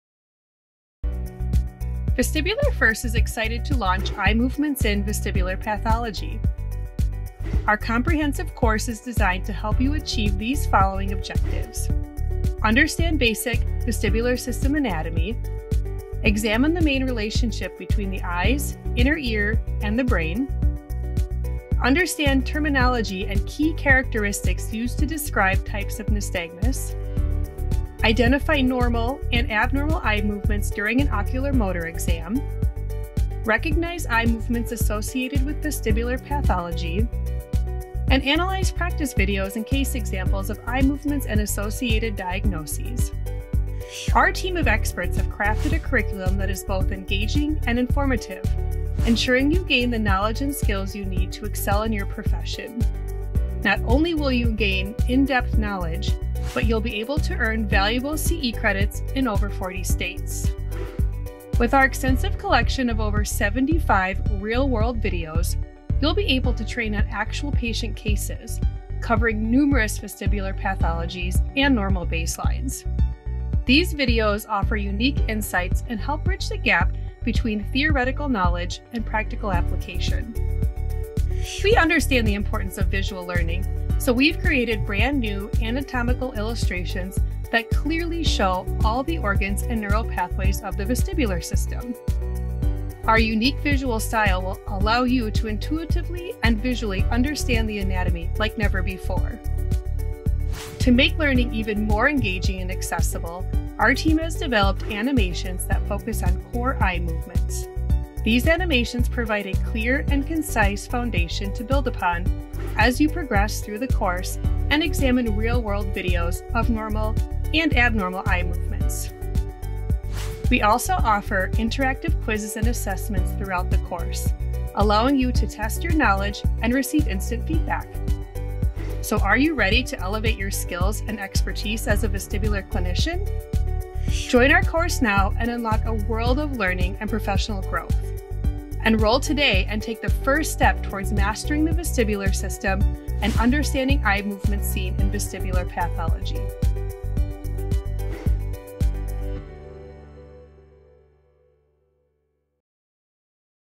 Eye Movements in Vestibular Pathology
Learn to observe and define the six main characteristics of eye movements seen in vestibular pathology. This course is perfect for beginner and intermediate vestibular clinicians--or anyone interested in learning how your inner ear helps the body maintain balance and gaze stability during movement.